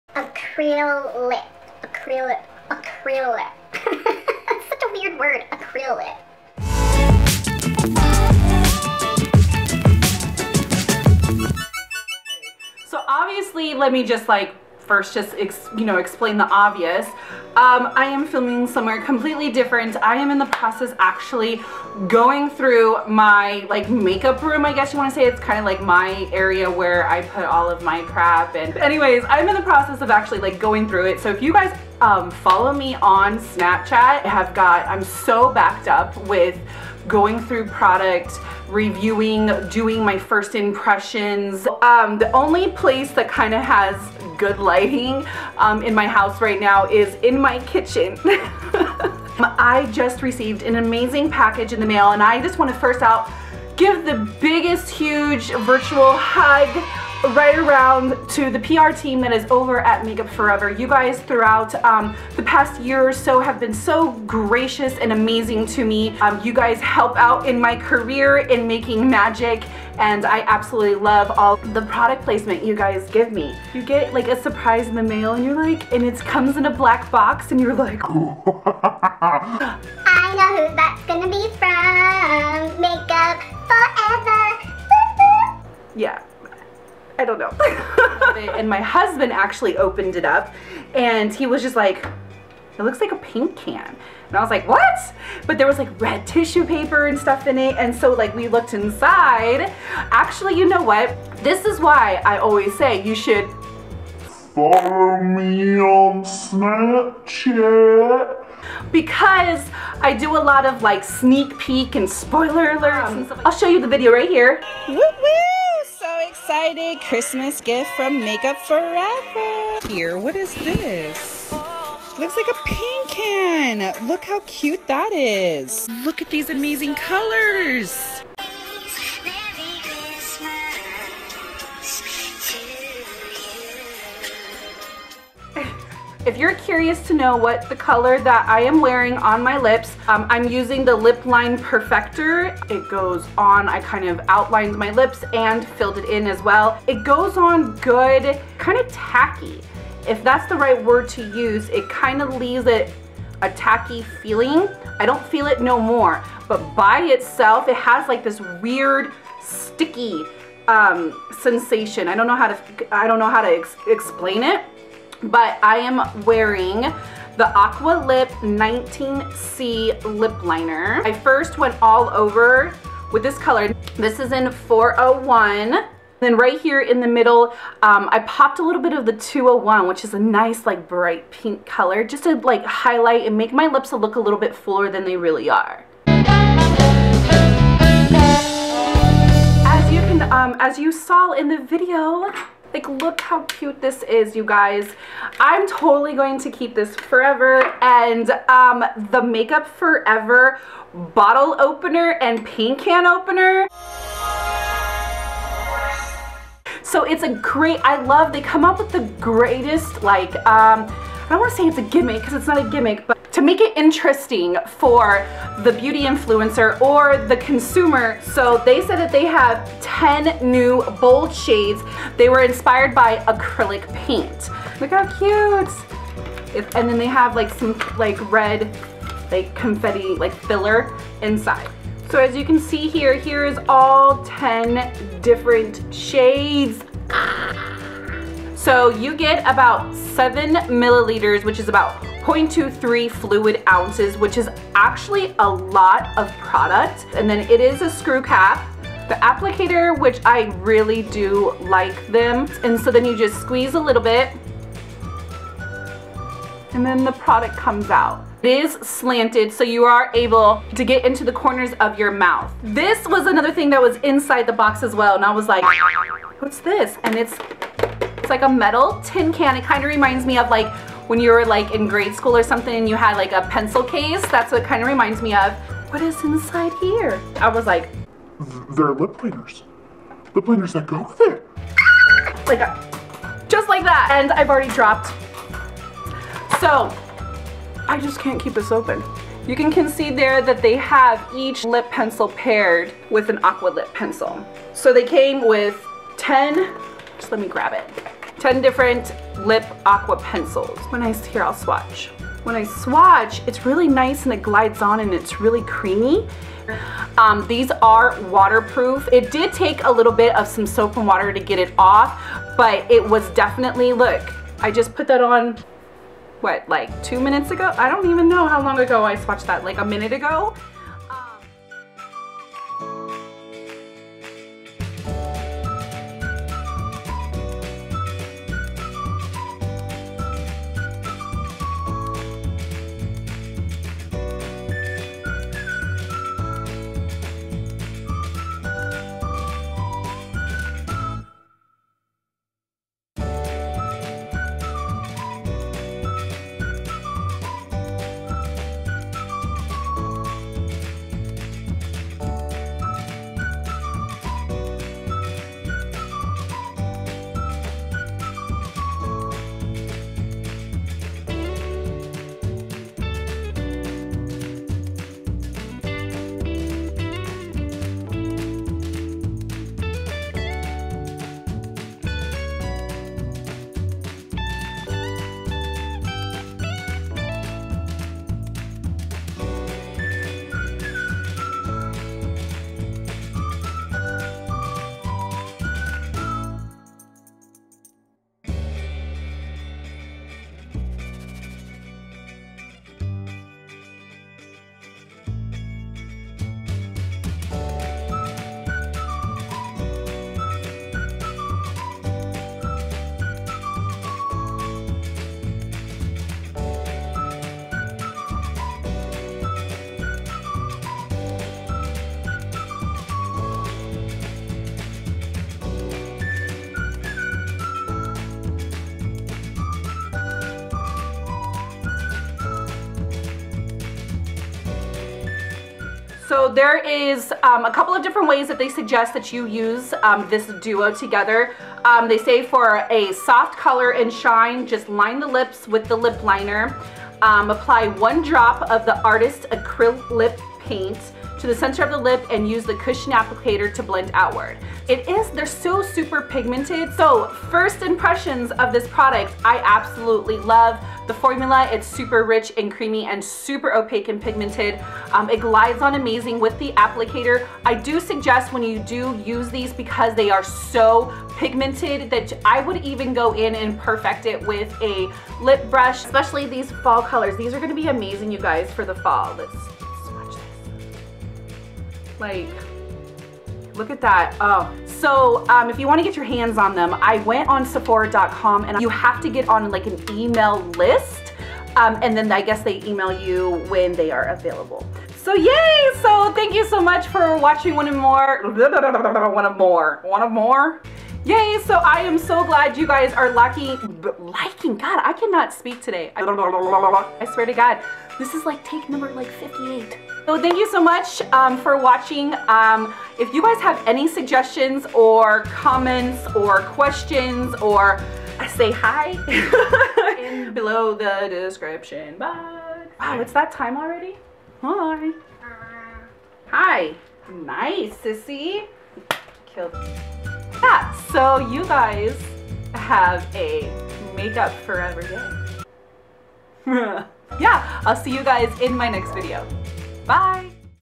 MUFE Artist Acrylip Full Collection | FIRST IMPRESSIONS + SWATCHES
**WATCH in HD** OH EM GEE Musician Jessie J & MAKE UP FOR EVER Teamed up for their *NEW* Artist collection for 2017!!This Collaboration couldn't be more PERFECT! This is the first of 3 Collections Jessie J did with MUFE.
I was gifted the most amazing package with ALL 10 shades of ACRYLIP & their matching AQUA LIP liners.

LAUNCHES
Online December 20th
In-Store January 2017

WHAT IM WEARING:
FACE: waterblend Foundation (Y405)
CONTOUR: prosculptingpalette No30
BLUSH: secondskin Creme blush in Flamingo Pink (215)
GLOW: Pro Light Fusion highlighter No2 ✌🏼️
E/S: Amber Brown (1662) Taupe Platinum (D562) Golden Khaki (D320)
LINER: Matte Dark Brown (M-60) Metallic Bronze (ME-42)
LASHES: 2 coats of excessivelash Mascara
BROWS: AquaBrow in Light Brown (20)
LIPS: AquaLip lip liner (19C) Acrylip paint (400)

SWEATER: TORRID